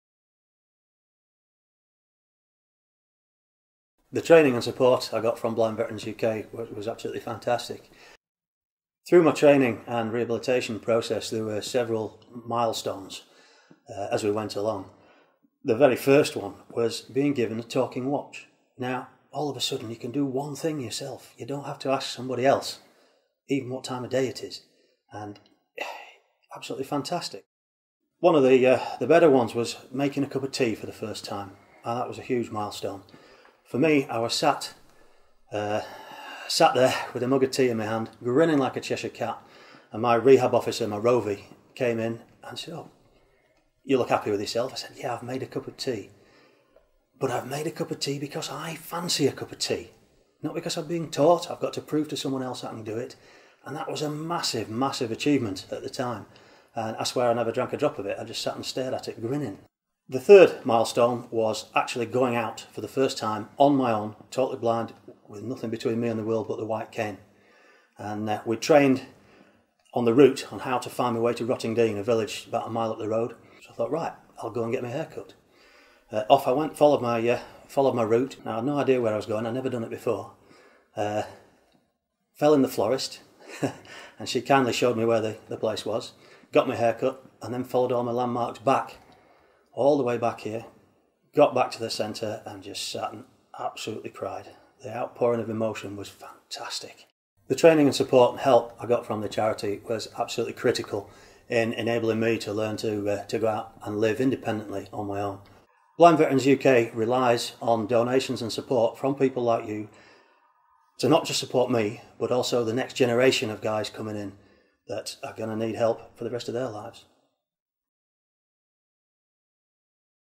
Mark Threadgold's story
Mark Threadgold receives free support from national charity Blind Veterans UK, which means he was able to make a cup of tea for the first time since losing his sight.